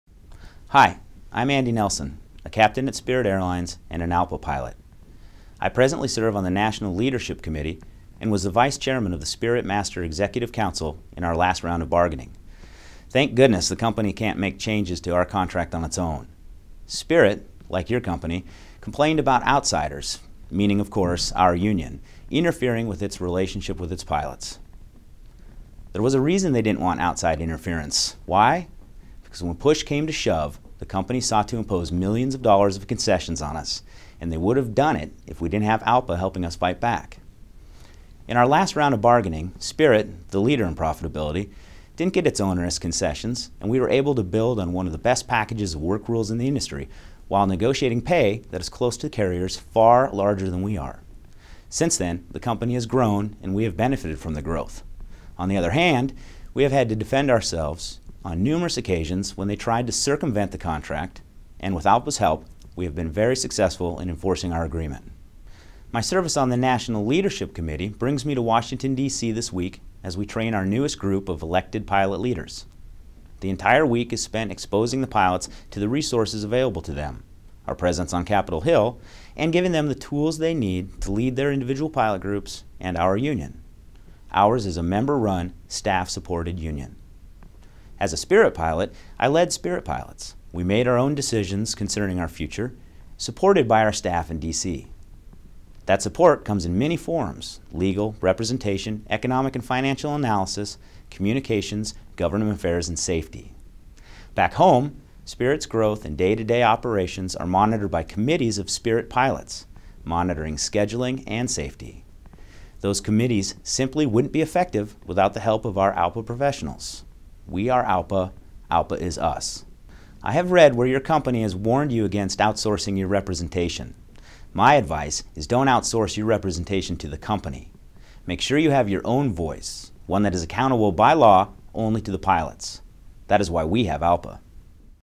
Spirit Pilots Support JetBlue Organizing
Capt. Andy Nelson addresses JetBlue pilots as the representation election approaches.